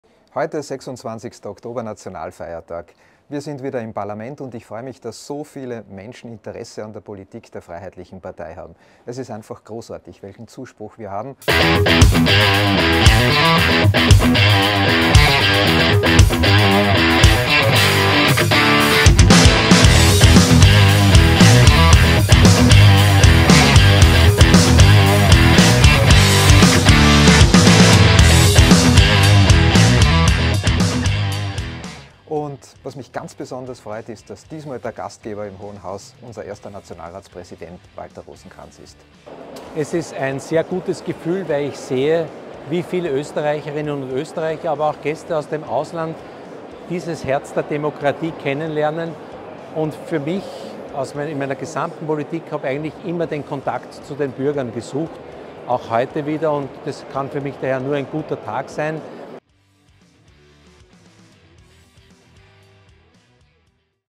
🇦🇹 Heimat & Neutralität im Herzen – der Nationalfeiertag 2024 mit der FPÖ! 🇦🇹
Der heutige Tag stand ganz im Zeichen unserer Heimat!🇦🇹 Beim „Tag der offenen Tür“ im Parlament konnten wir gemeinsam mit zahlreichen Bürgern unseres Landes den Nationalfeiertag feiern und ein starkes Zeichen für ein neutrales Österreich setzen.🇦🇹

Es war ein großartiger Tag voller wertvoller Begegnungen, spannender Gespräche und echter Gemeinschaft. Vielen Dank an alle, die diesen Tag mit uns verbracht und ihre Liebe zu Österreich zum Ausdruck gebracht haben! 📺 Hier ein paar Eindrücke von diesem besonderen Tag.